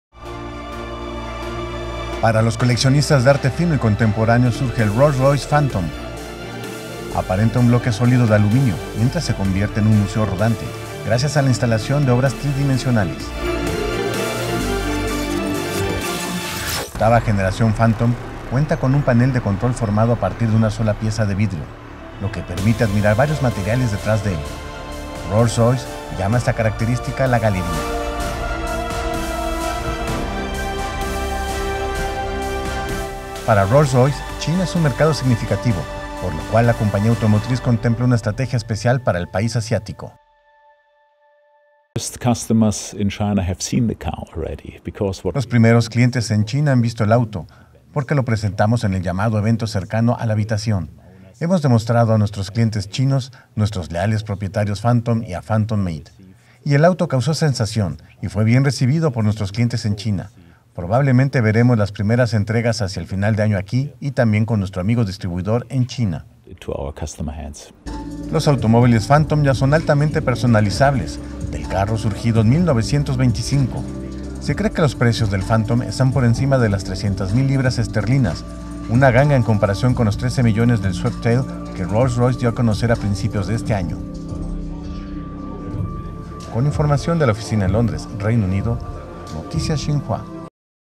New Rolls Royce Phantom, una galería de lujo rodante
Para los coleccionistas de arte fino y contemporáneo surge el Rolls-Royce Phantom. Aparenta un bloque sólido de aluminio, mientras se convierte en un museo rodante gracias a la instalación de obras tridimensionales.